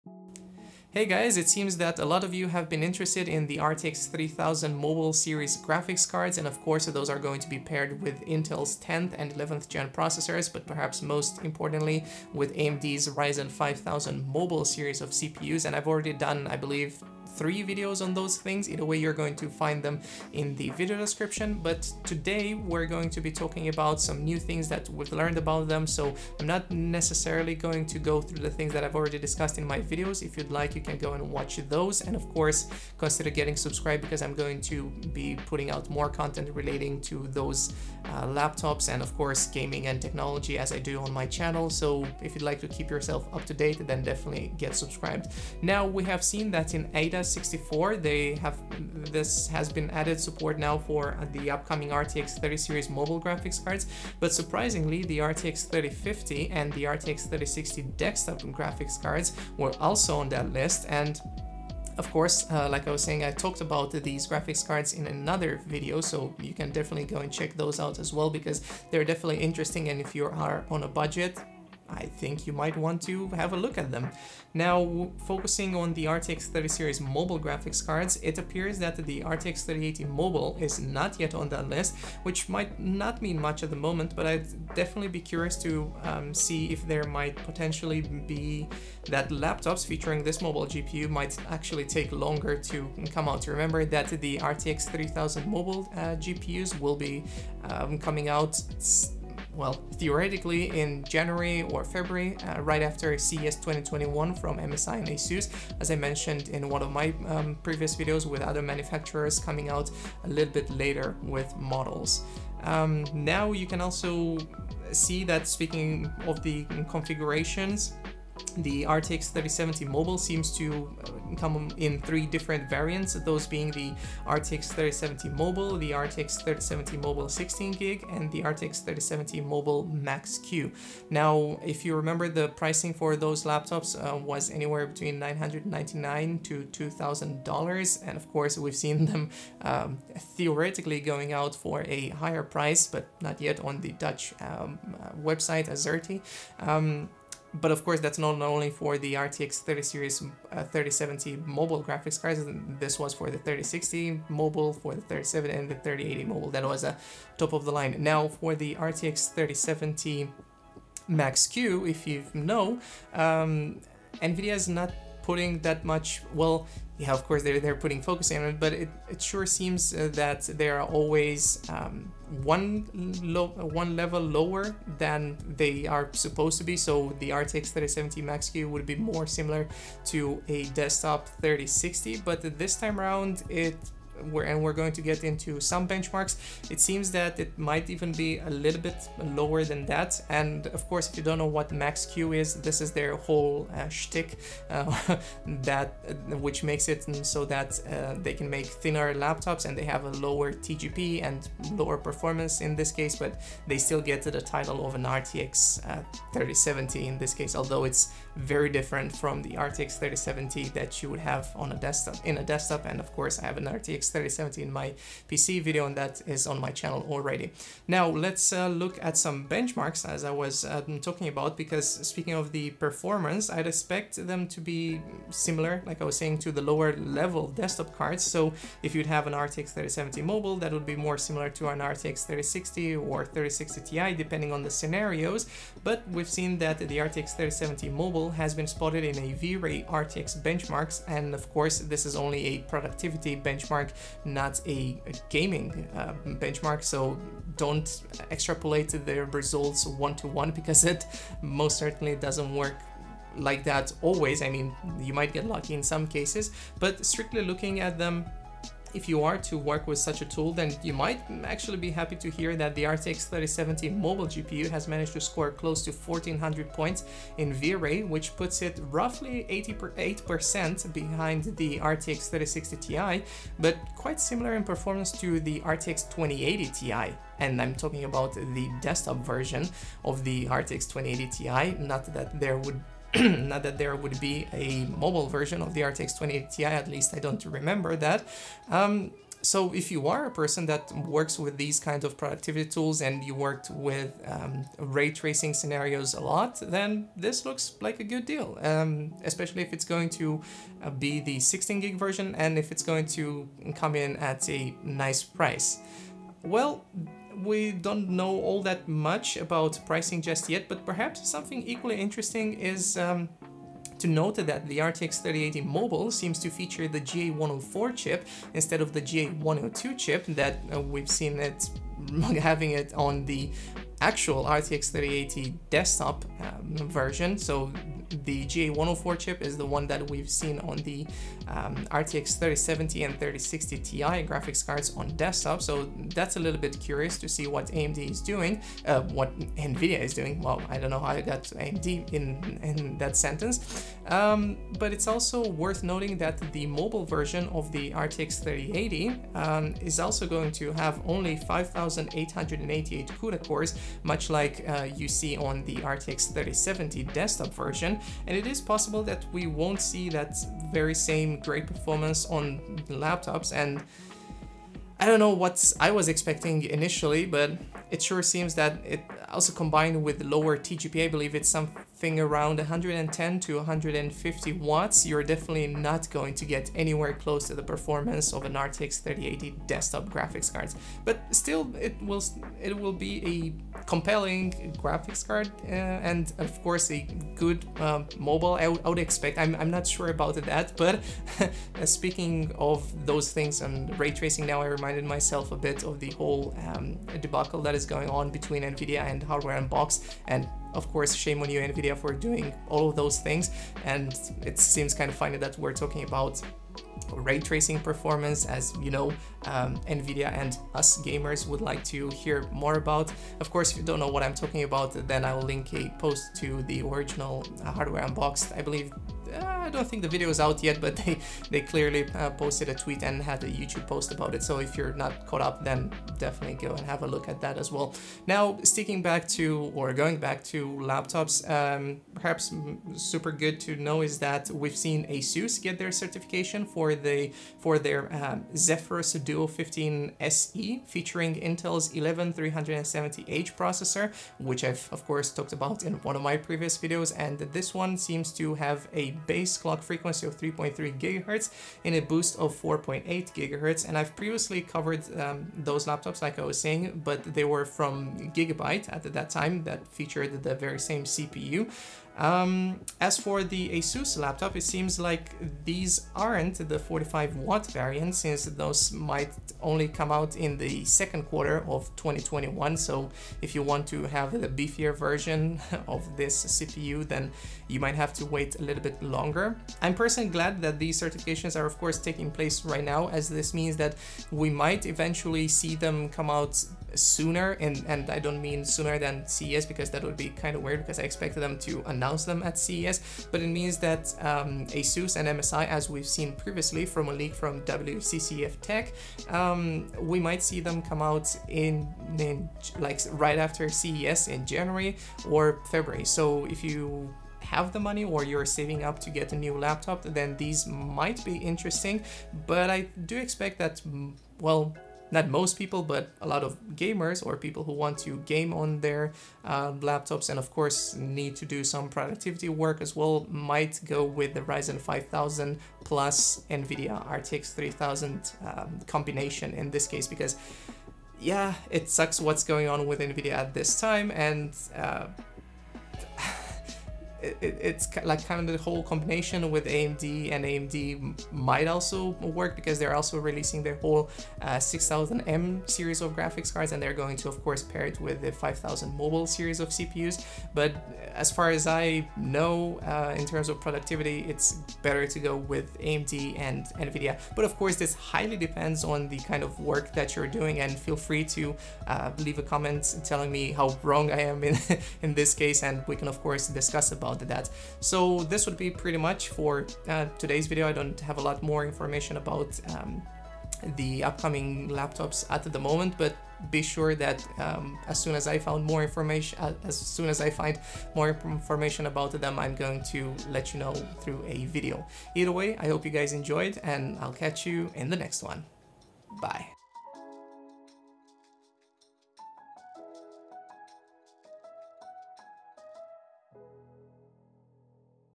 RTX 3000 mobile laptops are right around the corner, but are they good?
RTX 3000 Mobile GPUs - what can you expect from them? The next generation of laptops featuring NVIDIA's RTX 3000 graphics cards is right around the corner and we have seen the RTX 3070 mobile score similarly to an RTX 3060 Ti in a V-Ray benchmark, making it effective for people who would like to work and game on the go. The RTX 3080 Mobile is missing from the list, but we've seen Asus receive certifications for their Zephyrus Duo 15 SE and we've also seen more details about their Asus TUF RTX 3000 mobile series laptops. The RTX 3080 mobile GPU, however, would be based on the GA104 chip instead of GA102 as the desktop variant and it should also come with only 5888 CUDA cores (like the RTX 3070 desktop variant) and a cTGP of 110-150W, which is far lower than the desktop RTX 3080.

The RYZEN 5000 chips will be combined with the RTX 3000 series of GPUs in what I suspect will be some compelling (but pricey) laptops. Let's talk about them!

RTX 3070 mobile benchmarked and more RTX 3000 laptops are coming soon

Timestamps:

0:00 - RTX 3000 mobile GPUs are coming, paired with AMD 5000 and Intel 10 and 11th gen
0:45 - RTX 3070 mobile found in AIDA64
1:05 - RTX 3080 Mobile not on the list & RTX 3080Ti mention
1:40 - RTX 3000 series release date and RTX configurations
2:00 - RTX 3070, 3070 16GB and 3070 Max-Q
2:50 - RTX 3070 Max Q explanation and benchmark
4:25 - RTX 3070 mobile vs RTX 3060 Ti desktop
5:00 - RTX 3000 Prices and RTX 3080 on GA104 chip instead of GA102
6:31 - NVIDIA vs Hardware Unboxed: Ray tracing debacle
7:08 - ASUS ROG Zephyrus DUO 15 SE with RTX 3000 and i7-11370H
7:30 - Gigabyte Aero with RTX 3060, 3070
7:41 - Intel 11th gen ultra low power APUs & 11th gen CPUs
8:00 - Asus certifications for RTX 30-series laptop
8:20 - Asus and MSI to release RTX 3000 laptops in January
8:43 - Which laptop should you go for?